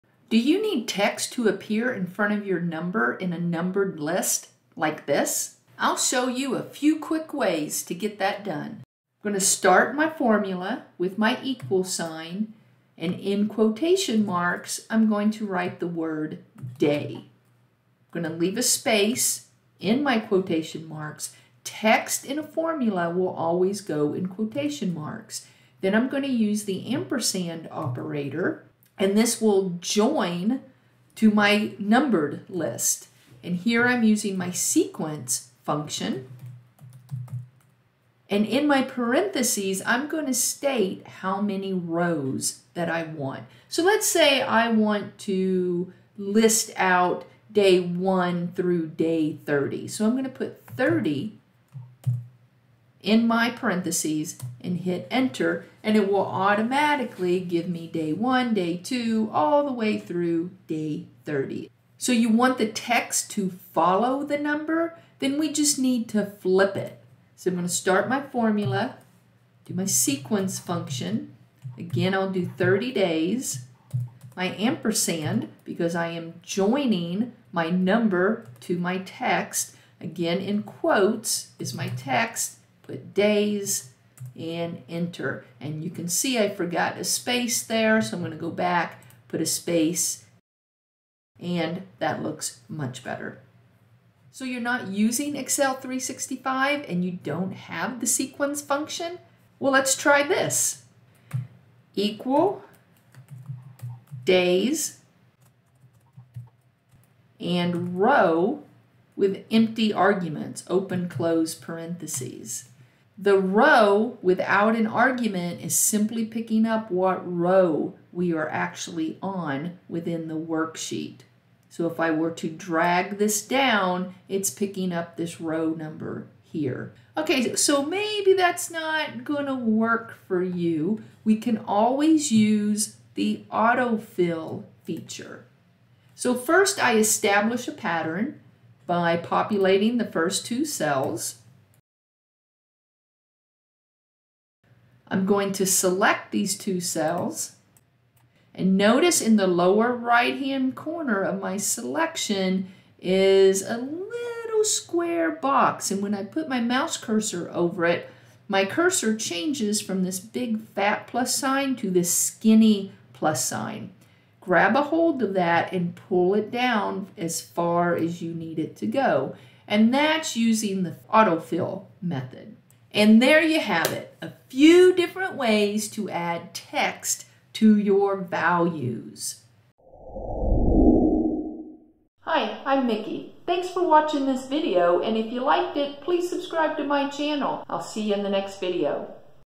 How to add text to a numbered list
Do you need text at the beginning or end of a set of values?
Something like this?
Day 1, No. 1 or Exhibit 1
I’ve got a few ways to show you in this video.
I'll show you the SEQUENCE function for Excel 365. If you are not using Office 365, then I have a couple of methods I demonstrate in the video.

Contents:
0:09 Using the SEQUENCE function
1:36 ROW()
2:10 Autofill